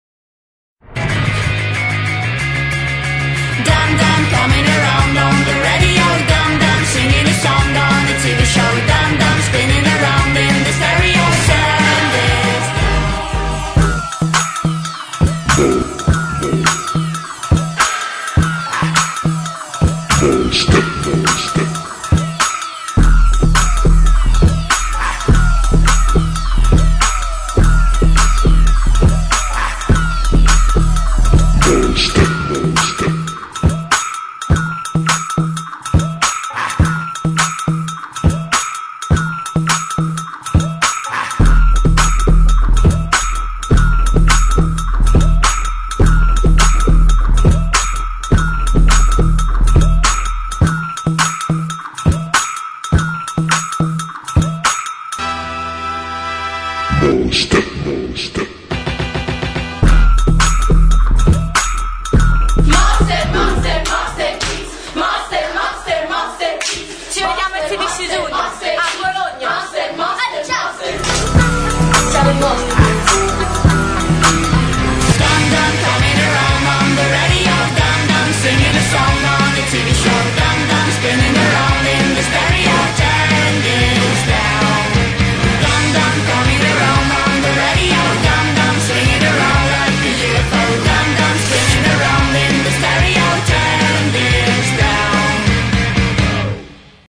Monster Kids for the Jambo
The JamBO è il primo Urban Summer Festival d'Italia. 3 giorni di musica elettronica ed action sport, ad alto tasso adrenalico. Gare ed esibizioni di fmx, skateboard, calcio e basket freestyle, parkour, wakeboard e surf con le icone mondiali del freestyle.
14-15-16 giugno 2013 Bologna Fiere